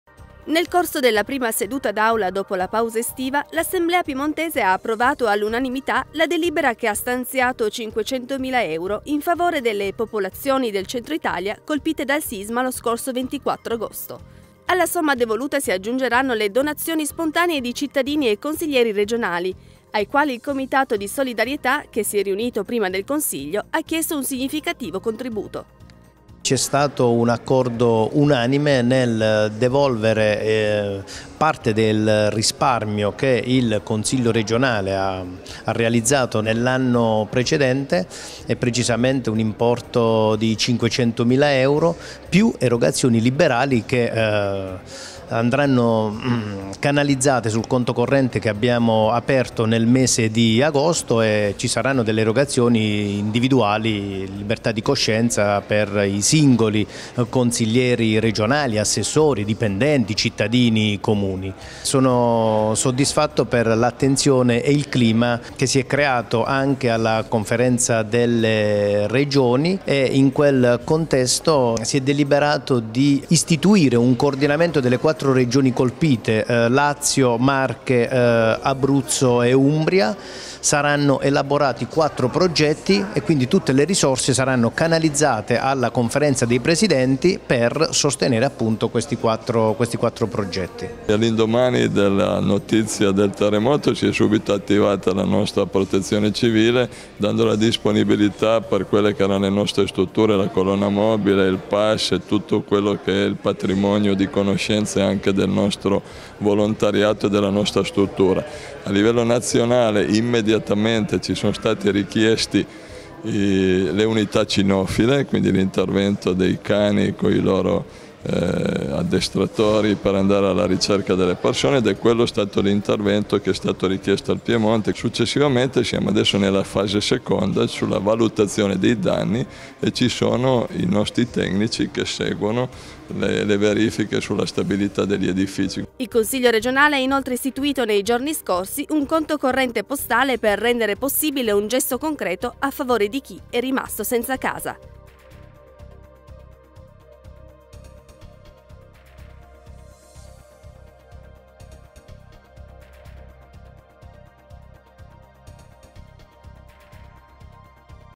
Il Consiglio regionale per i terremotati
Nel corso della prima seduta d’Aula dopo la pausa estiva, l’Assemblea piemontese ha approvato all’unanimità la delibera che ha stanziato cinquecentomila euro in favore delle popolazioni del centro Italia colpite dal sisma lo scorso 24 agosto. Alla somma devoluta si aggiungeranno le donazioni spontanee di cittadini e consiglieri regionali, ai quali il Comitato di solidarietà, che si è riunito prima del Consiglio, ha chiesto un significativo contributo.

Interviste a:
Mauro Laus, presidente del Consiglio regionale del Piemonte
Alberto Valmaggia, Assessore alla Protezione Civile della Regione Piemonte

Il Consiglio regionale ha inoltre istituito nei giorni scorsi un conto corrente postale per rendere possibile un gesto concreto a favore di chi è rimasto senza casa.

Per donare:

IBAN IT-47-Z-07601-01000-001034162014 – Consiglio regionale del Piemonte - pro sisma Italia centrale servizio tesoreria.